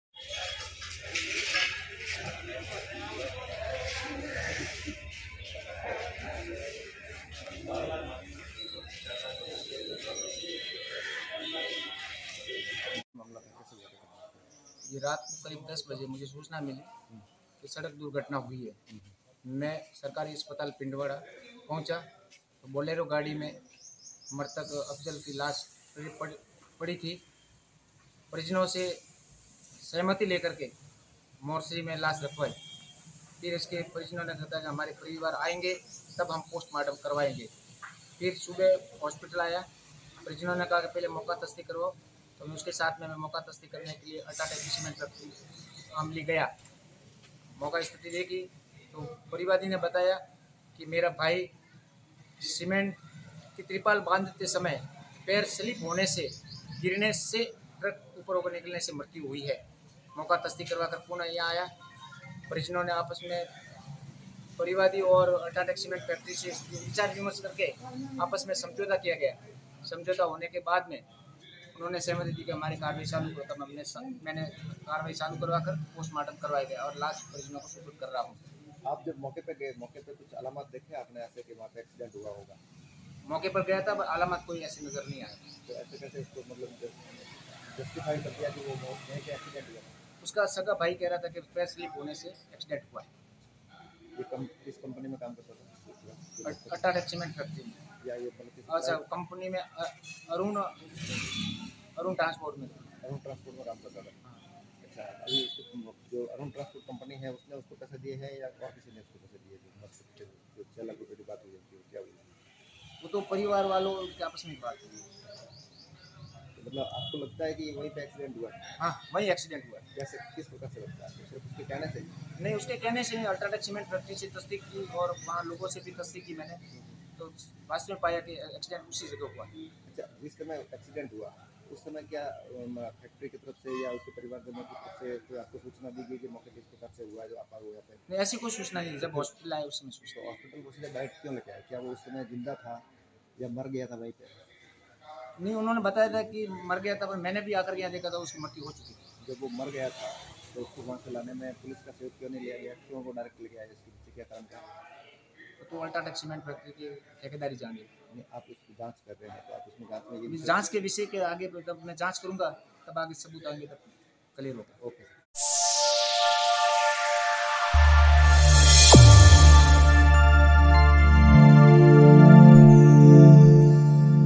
पिंडवाड़ा थाना क्षेत्र में हुआ हादसा, पुलिस ने शव को कब्जे में लेकर मोर्चरी में रखवाया | Sirohi News
About Video:

पिंडवाड़ा थाना क्षेत्र में हुआ हादसा, पुलिस ने शव को कब्जे में लेकर मोर्चरी में रखवाया | Sirohi News | 30 June 2023

सिरोही के पिंडवाड़ा से खबर,ट्रक के टायर के नीचे सिर आने से युवक की हुई दर्दनाक मौत,बारिश में ट्रक पर तिरपाल लगाते समय नीचे गिरा था युवक,नीचे गिरते ही ट्रक के टायर के नीचे आया सिर,ठेकेदार युवक को लेकर पहुंचे पिंडवाड़ा अस्पताल,सूचना पर ASI सोमाराम मीणा मय जाब्ता पहुंचे मौके पर,पुलिस ने शव को कब्जे में लेकर मोर्चरी में रखवाया,राजसमंद जिलां निवासी अफजल की हुई मौत,पिंडवाड़ा थाना क्षेत्र में हुआ हादसा | 30 June 2023